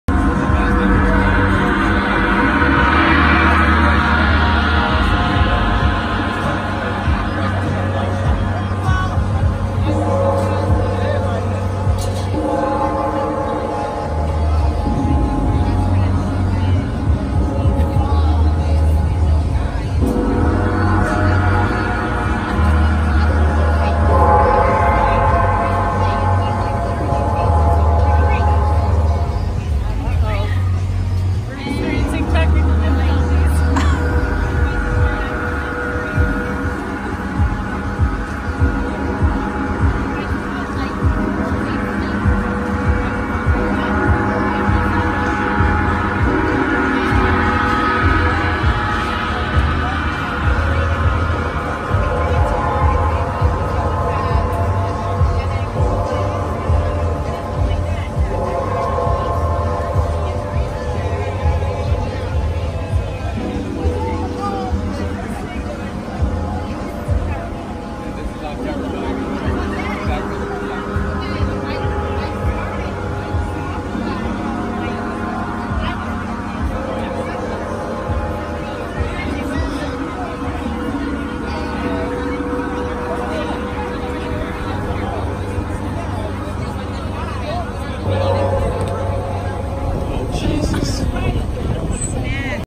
Gong Sandwich at the Newburgh Illuminated Festival
Imagine what it feels like to bathe in the healing sound vibrations of the gong from head to toe!